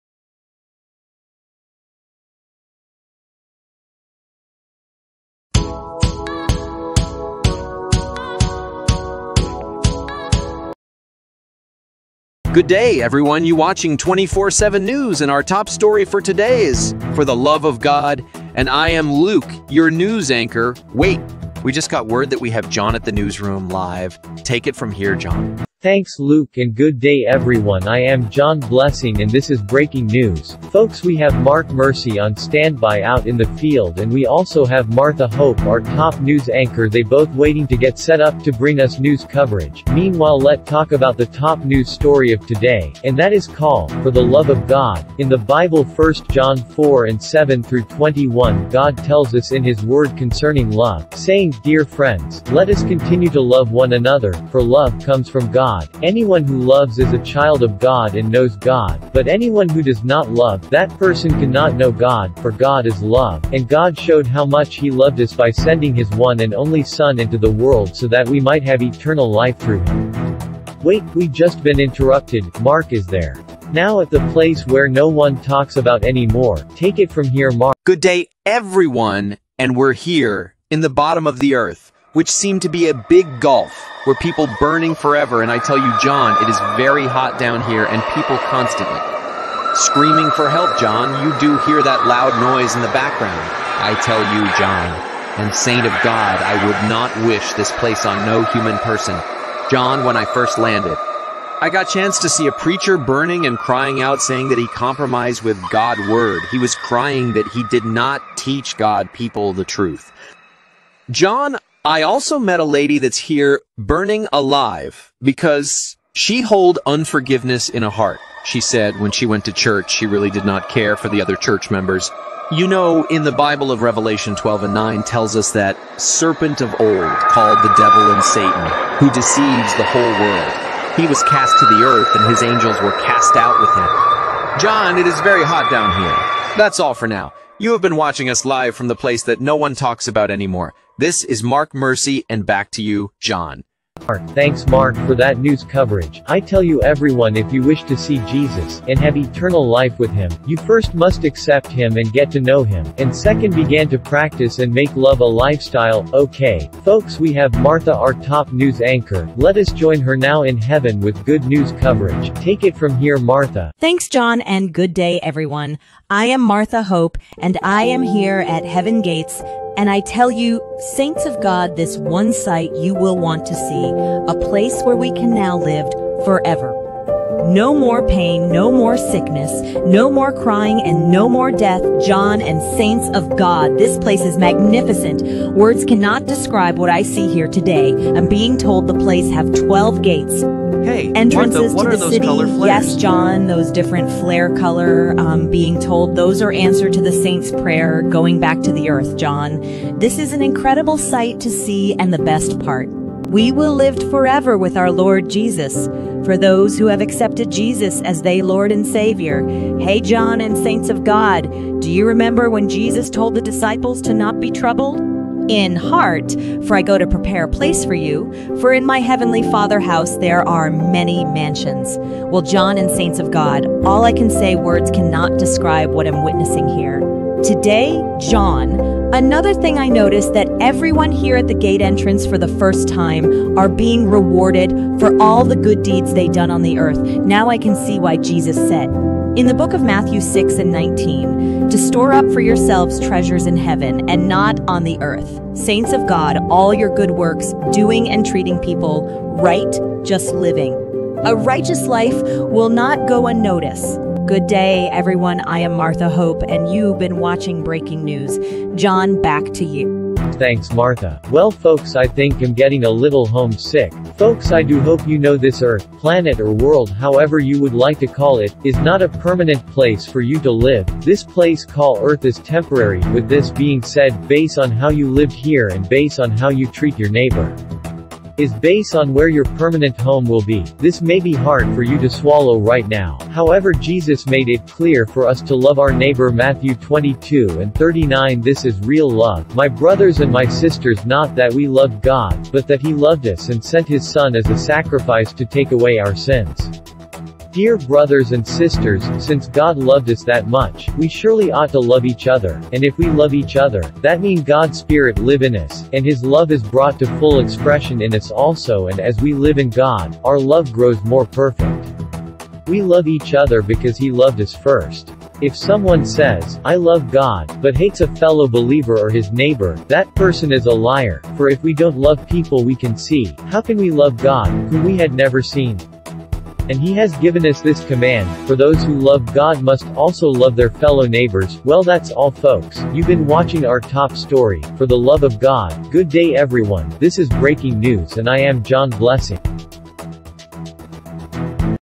For The Love of God / Rod Anderson
Video talks about God love

Thanks Luke and Good Day Everyone I am john Blessing and this is Breaking News ,, Folks we have Mark Mercy on standby out in the field and we also have Martha Hope our top news anchor they both waiting to get set up to bring us news coverage , mean while lets talk about the top News Story of today ,and that is call , for the love of God , in the bible first john 4 and 7 through 21 God tells us in his word concerning love, saying , Dear friends, let us continue to love one another, for love comes from God , Anyone who loves is a child of God and knows God , But anyone who does not love , that person can not know God, for God is love , and God showed how much he loved us by sending his one and only Son into the world so that we might have eternal life through him. Wait , we just been interrupted , Mark is their now at the place where no one talks about any More , take it from here Mark , thanks Mark for that News coverage , I tell you everyone if you wish to see Jesus , and have eternal life with him , you first must accept him and get to know him ,and second began to practice and make love a lifestyle , ok , folks we have Martha our top News Anchor , let us join her now in heaven with Good News coverage , take it from here Martha, Hey Martha what are those color flares ? thanks Martha , well Folks I think am getting a little home sick , folks I do hope you know this earth , planet or world however you would like to call it , is not a permanent place for you to lived , this place call earth is temporary , with this being said , base on how you lived here and base on how you treat your neighbor is base on where your permanent home will be , this maybe hard for you to swallow right now , However Jesus made it clear for us to love our neighbor Matthew 22 and 39 This is real love ,My brothers and My sisters not that we loved God, but that he loved us and sent his Son as a sacrifice to take away our sins. Dear brothers and sisters , since God loved us that much, we surely ought to love each other , and if we love each other, that mean God spirit live in Us , and his love is brought to full expression in Us also And as we live in God, our love grows more perfect. We love each other because he loved us first. If someone says, "I love God," but hates a fellow believer or his neighbor , that person is a liar , for if we don't love people we can see, how can we love God, whom we had never seen? And he has given us this command: For those who love God must also love their fellow neighbors , well that's all folks ,you been watching our top story , for the love of God , good day everyone , this is breaking news and I am John Blessing ,,